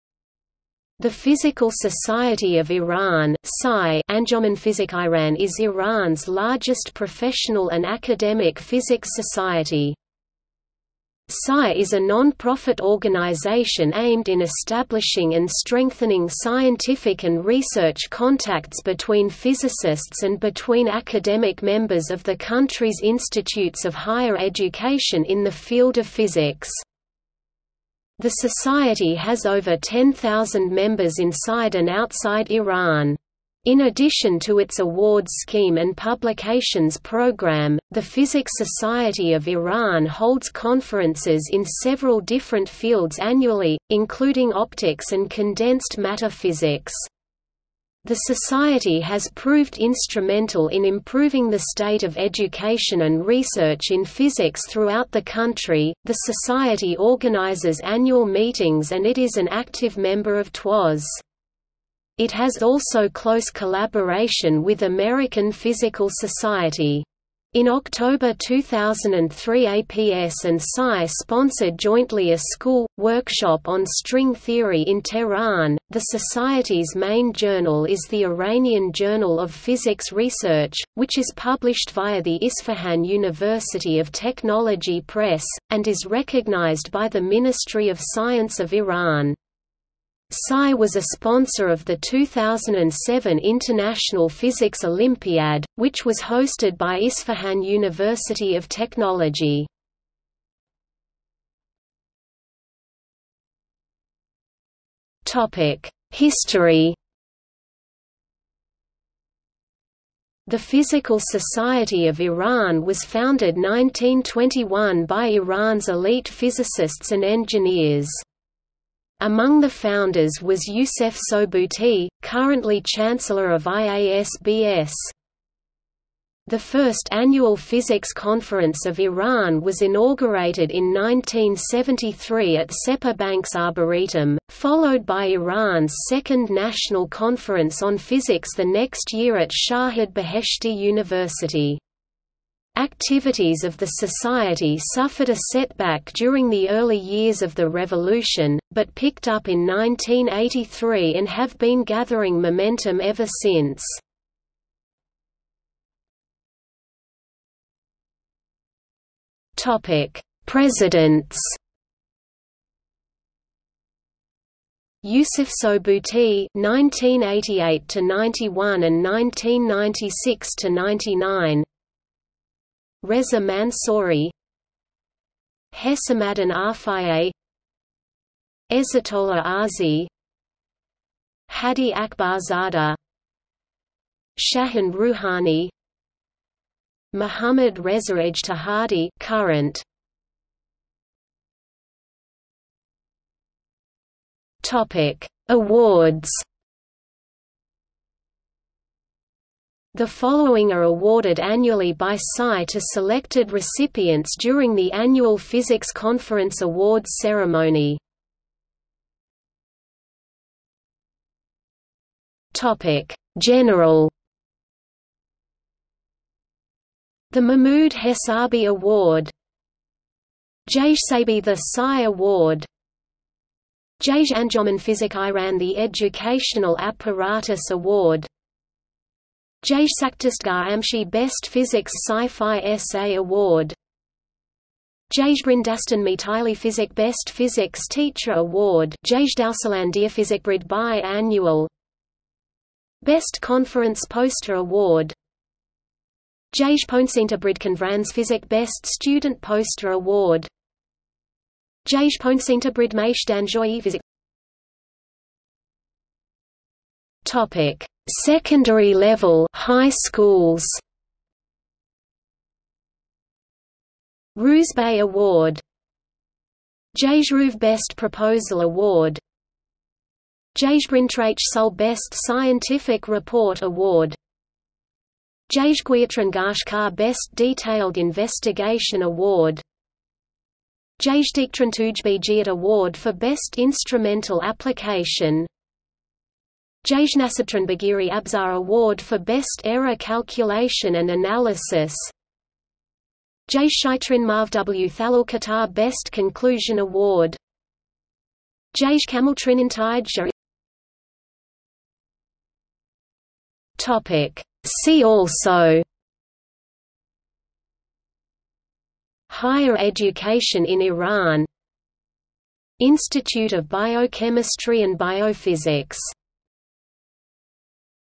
The Physical Society of Iran | Wikipedia audio article
00:01:41 1 History
00:02:36 2 Presidents
00:03:11 3 Awards
00:03:29 3.1 General
00:04:14 3.2 Secondary Level (high schools)
00:05:00 4 See also

- Socrates

SUMMARY
The Physical Society of Iran (PSI) (انجمن فيزيک ايران) is Iran's largest professional and academic physics society.
PSI is a non-profit organization aimed in establishing and strengthening scientific and research contacts between physicists and between academic members of the country's institutes of higher education in the field of physics.
The society has over 10,000 members inside and outside Iran. In addition to its awards scheme and publications programme, the Physics Society of Iran holds conferences in several different fields annually, including optics and condensed matter physics. The society has proved instrumental in improving the state of education and research in physics throughout the country.The society organizes annual meetings and it is an active member of TWAS. It has also close collaboration with American Physical Society. In October 2003 APS  and PSI sponsored jointly a school/workshop on string theory in Tehran.The society's main journal is the Iranian Journal of Physics Research, which is published via the Isfahan University of Technology Press, and is recognized by the Ministry of Science of Iran. PSI was a sponsor of the 2007 International Physics Olympiad, which was hosted by Isfahan University of Technology.